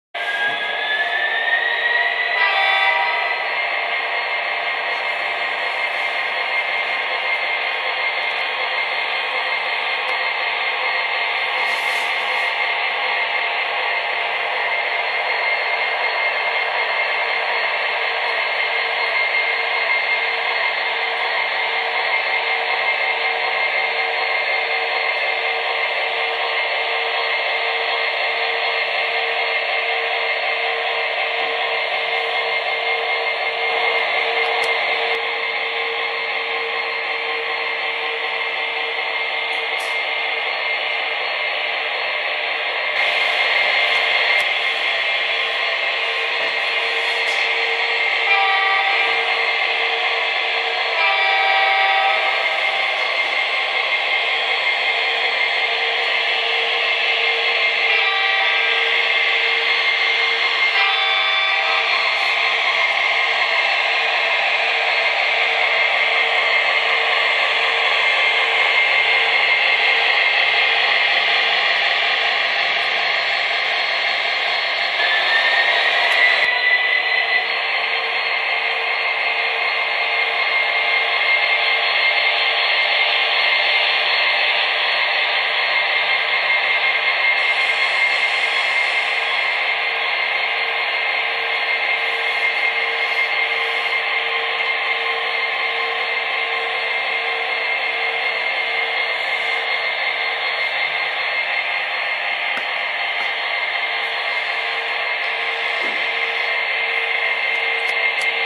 Building HO Layout
Building HO Layout. Testing the locomotive by MTH SD70Ace Rio Grande DCC trains nce power cab scenery mountains yard woodland scenics whalters